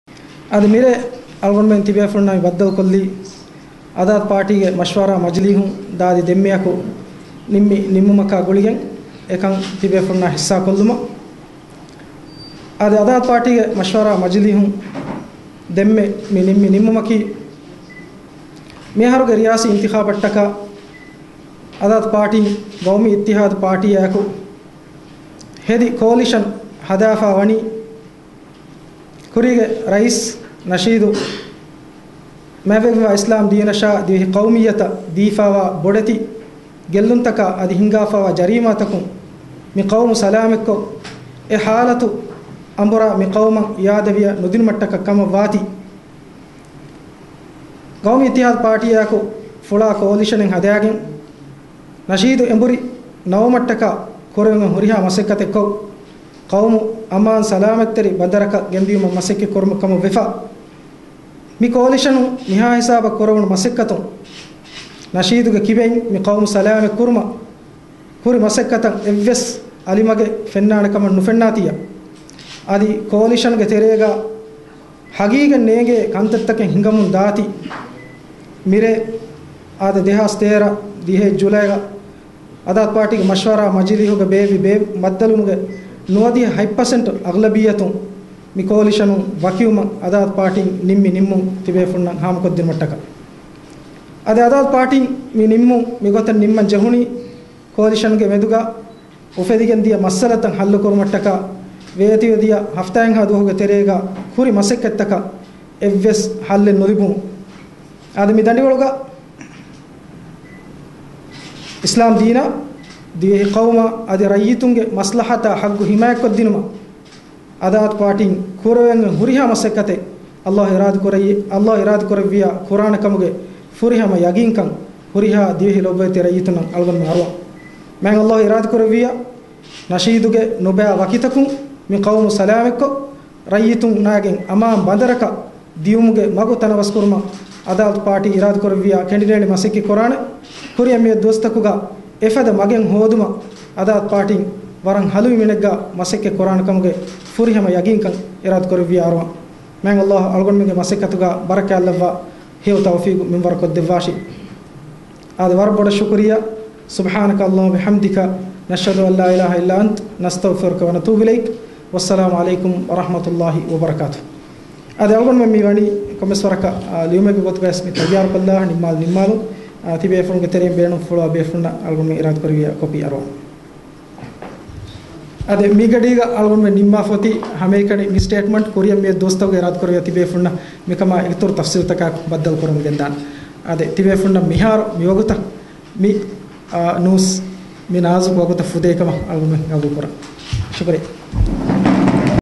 Adhaalath Party press conference
Adhaalath Party announces it has severed coalition ties with President Mohamed Waheed at a press conference on July 10, 2013. Haveeru video: Mohamed Sajid.